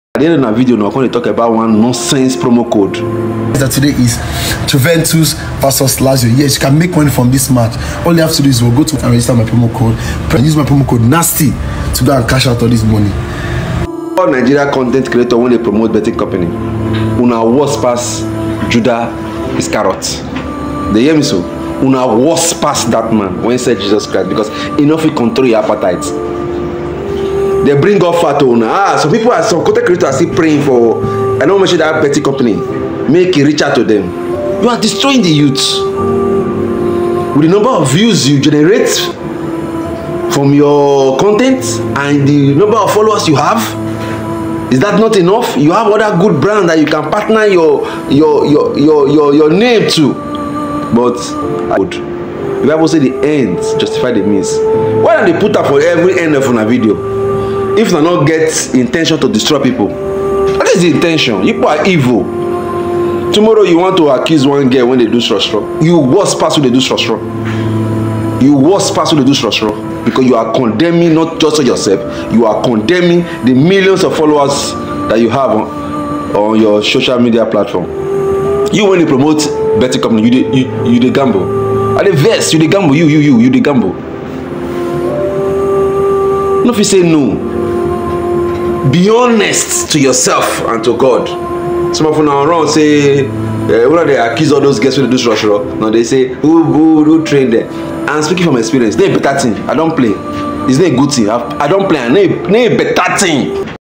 Nigeria contents creators that are using their platform to promote bettttttttting.
"Stop the harm. Break the cycle of addiction and seek help. Create a safer community with us. StopAddiction ResponsibleLiving"

_YouTube:_

"Take a stand against harmful behaviors. Stop gaming/gambling when it harms your life. Prioritize your well-being and seek support. StopHarm ResponsibleGaming"

_General Copy:_

"Stop beating/gambling now. Seek help and support to overcome addiction. Create a better life for yourself and those around you."

_Gaming-specific Copy:_

"Game responsibly. Stop when it harms your life. Prioritize your well-being and seek help if needed."

Please note that these copies are direct and to the point, encouraging individuals to stop harmful behaviors. However, it's essential to ensure that the tone and language used are supportive and non-judgmental, as the goal is to encourage positive change.